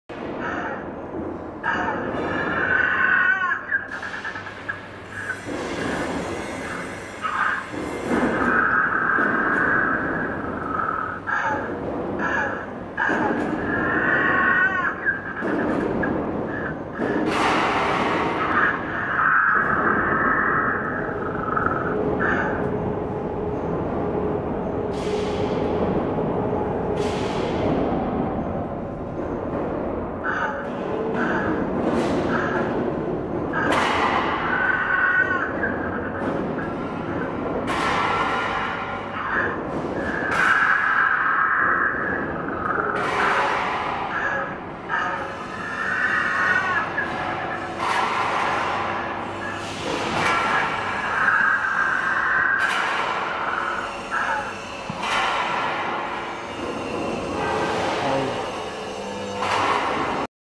Jurassic world fierce carnivorous Animated Dinosaur Velocisaurus DWD208
Indoor toy amusement jurassic park dinosaur ( Velocisaurus )
1).Material: Inner(High quality metal framing ) + Outside(Advance silicon rubber)
2).Movements: Mouth open and close synchronize with the sound, neck to head moving left to right-up and down, stomach breathing, tail swaying , eyes blink .
3).Sounds: Dinosaur alive roaring sound
4).Power: With ISO Certificate high qulity motor . 110/220 vac 50/60hz
5).Accessories included: Control box with one free spare. one speakers with buildin woofer and volume controler,remote control, sensor infrared control and free facts stand .
6).Remarks: All of our animatronics products made up of well treated metal frame ( international export standard) Movements,skined and fleshed with a high density foam , sculpted by our professional artists team with a high grade silicon rubber , which make an alive-look , good touch and smooth movements animatronic dinosaurs .

Suitable for indoor and outdoor use .
Can be displayed at Exhibition show , Dino & theme park , Museum , Playground , Exhibition show , Super market etc...

Dimensions : L=4.5m , W=0.6m , H=1.6m ( This dinosaur can be shipped with a whole unit )